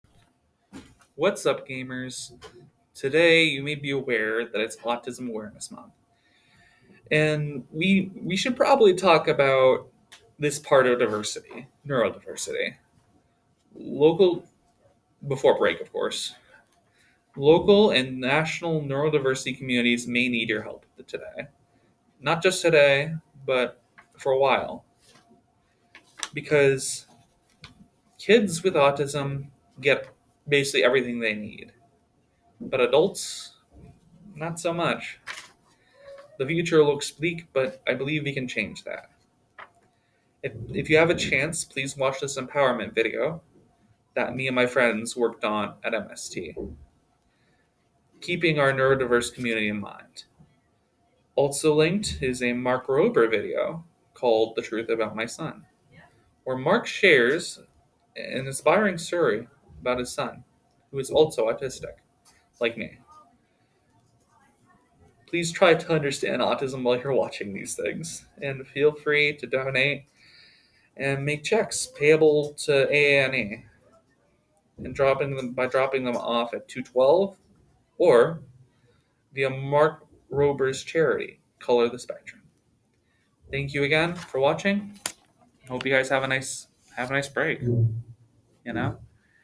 Colby Mandeville Autism Awareness Week
An introductory video to autism awareness week and how you can help.